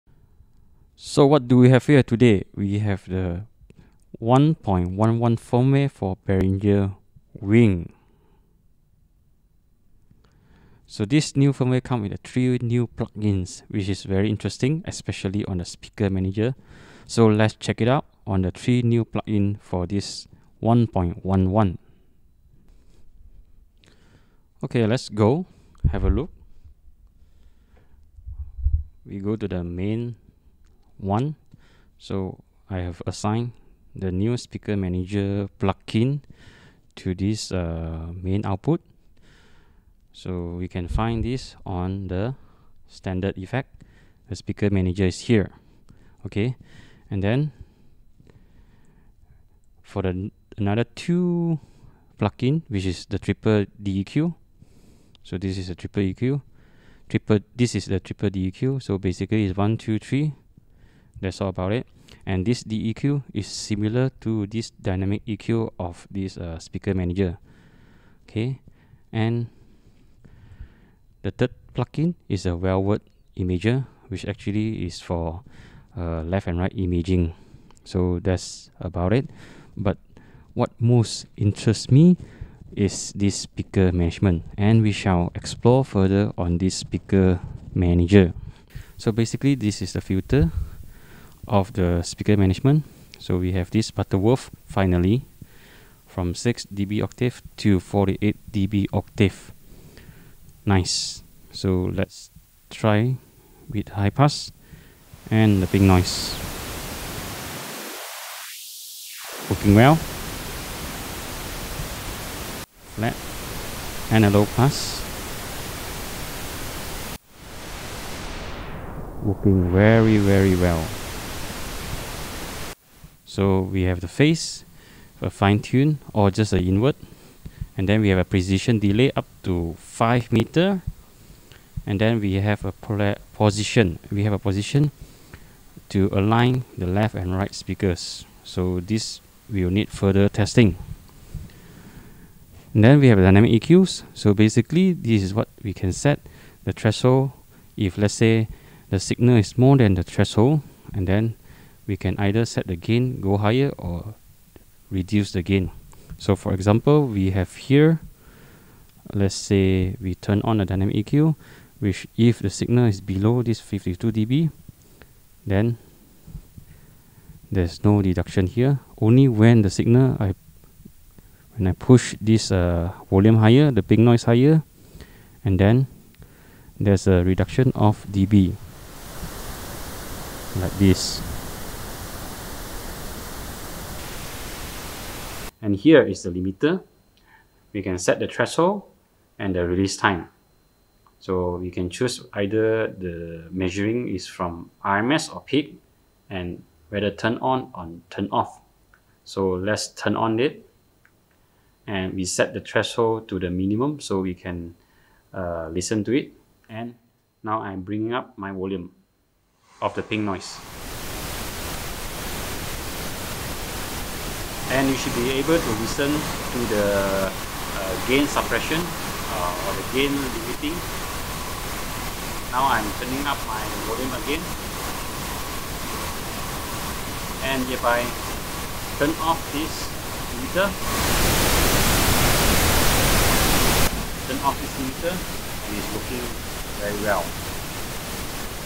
Speaker Manager Plugin aka Digital Loudspeaker Management inside Behringer WING (Firmware v1.11)
Firmware v1.11 of Behringer was a major upgrade where the developer give us Speaker Manager plugin. This is frequently known as outboard processor as Digital Loudspeaker Management System. With this powerful feature built-in this board, this board is one of the best value for money for now. No doubt.